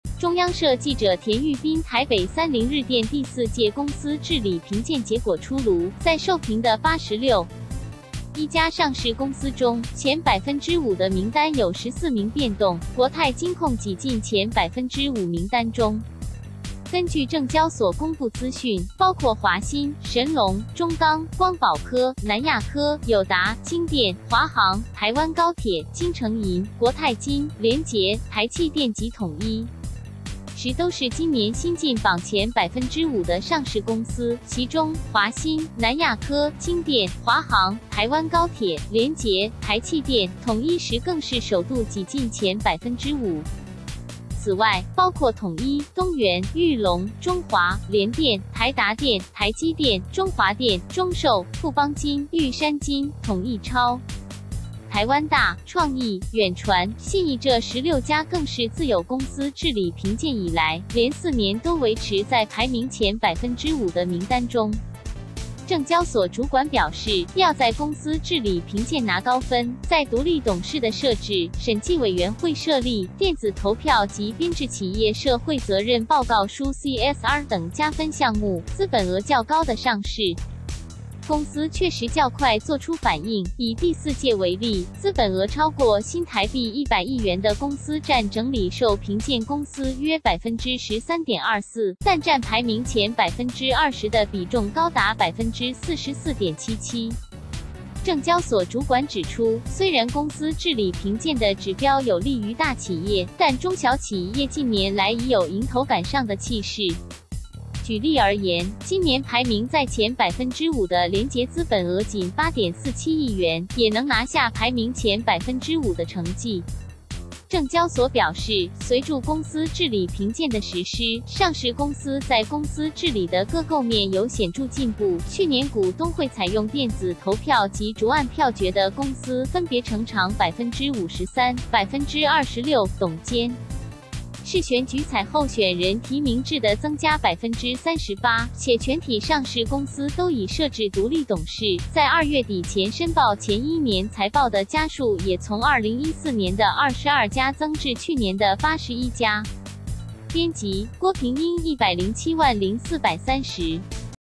公司治理評鑑結果 這14家公司擠進前5%
（中央社記者田裕斌台北30日電）第四屆公司治理評鑑結果出爐，在受評的861家上市公司中，前5%的名單有14名變動，國泰金控擠進前5%名單中。根據證交所公布資訊，包括華新、神隆、中鋼、光寶科、南亞科、友...